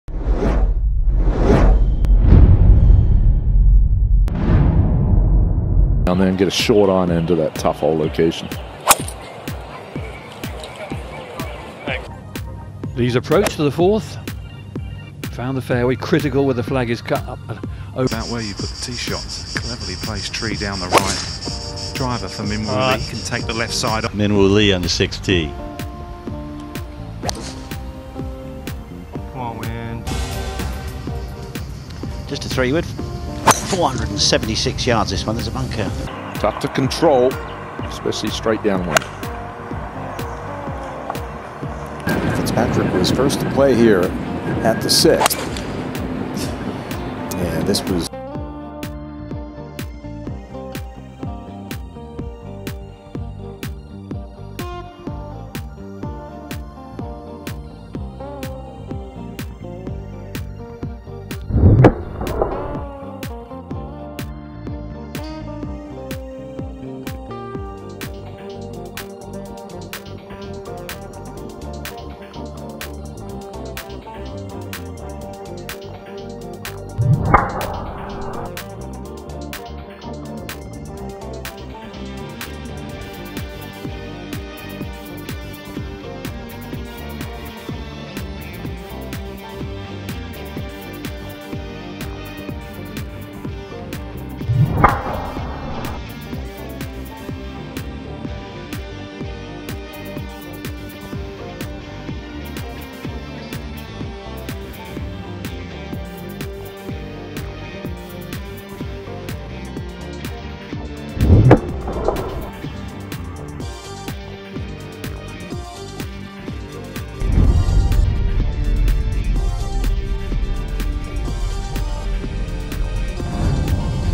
Distance King "Min Woo Lee" Amazing Lower Body Lag Swings & Slow Motions
Just Watching Beautiful Swing Can Help Your Golf Swing~!!